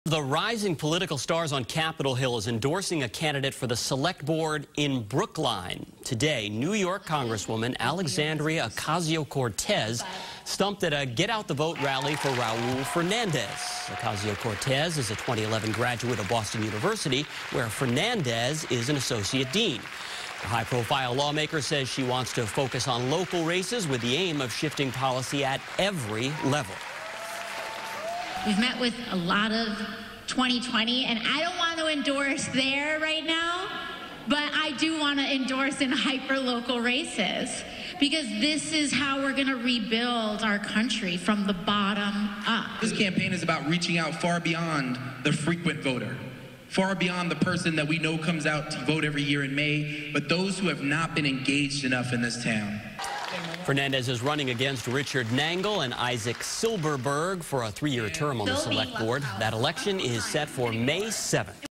AOC Campaigns In Brookline For Selectman Candidate Fernandez
Both AOC and Fernandez have a connection to Boston University.